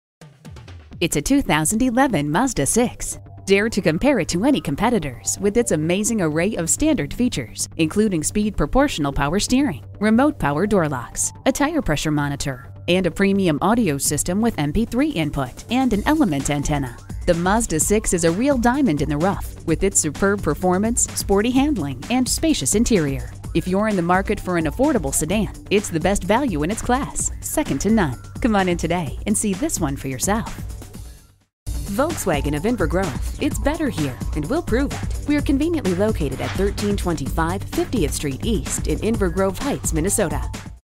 2011 Mazda MAZDA6 St-Paul MN Minneapolis, MN 68800P - SOLD
Year: 2011
Make: Mazda
Model: MAZDA6
Trim: 4dr Sdn Auto i Sport
Engine: 2.5 L 4 Cyl. Cyl.
Transmission: 5-Speed A/T
Color: Kona Blue Mica
Interior: Black
Mileage: 52964
Stock #: 68800P

Address:
1325 50th Street East